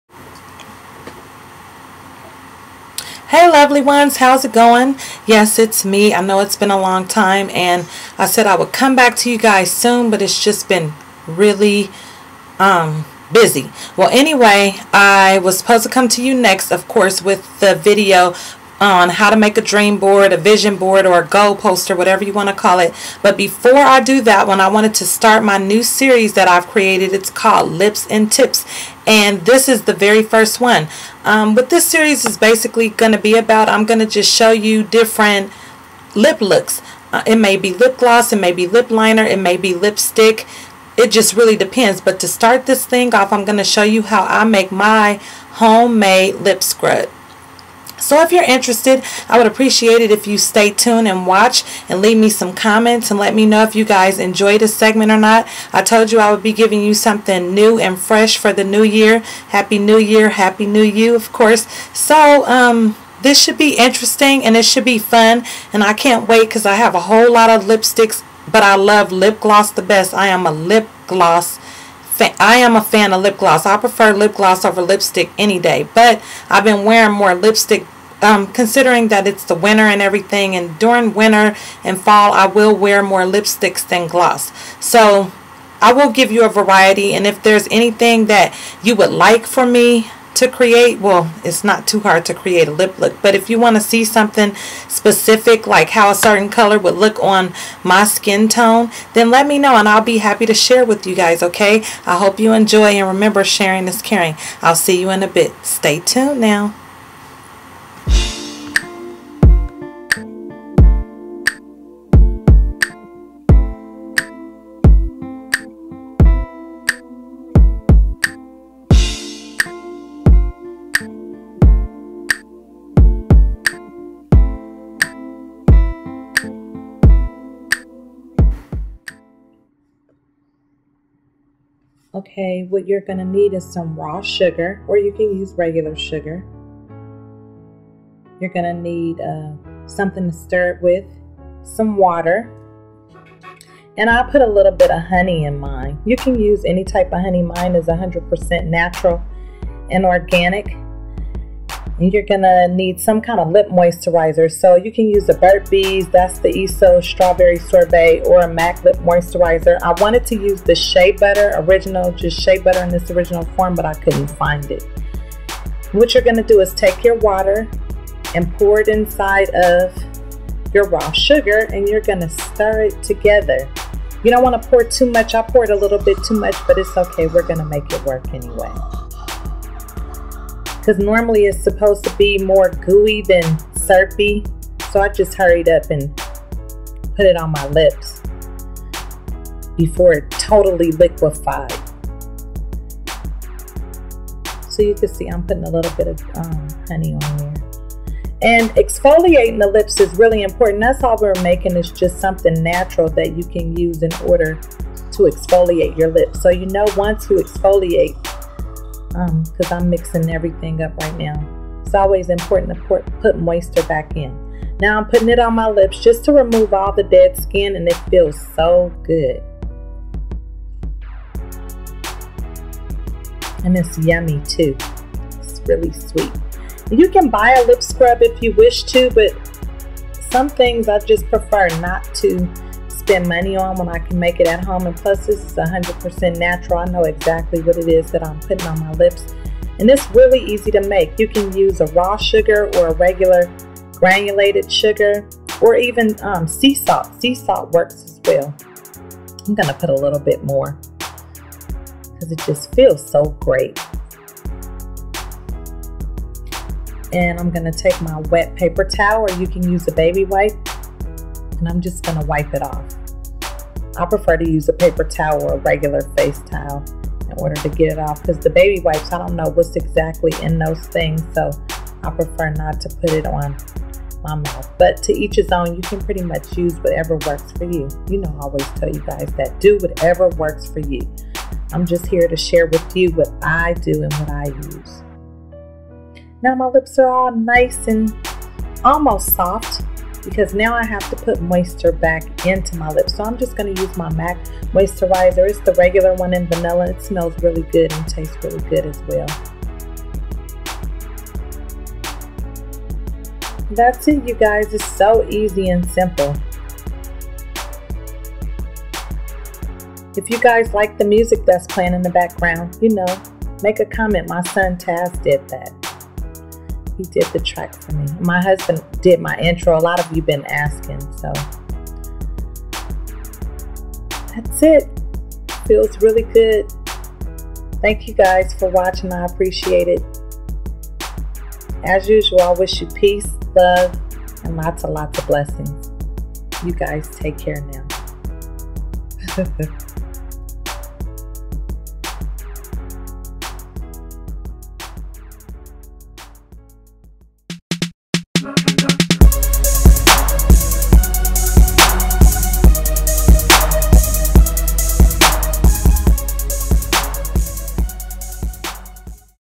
Lips and Tips Exfoliating
This series will help you to enhance the beauty of an important feature of your face The lips. You kiss with them smile with them so take care of them. This particular video I am showing you how to make a natural exfoliating serum. All you need is Raw Sugar Honey and Water that's all. I also featured the eos lipbalm burt bees lipbalm and mac lip moisturizer in this vid.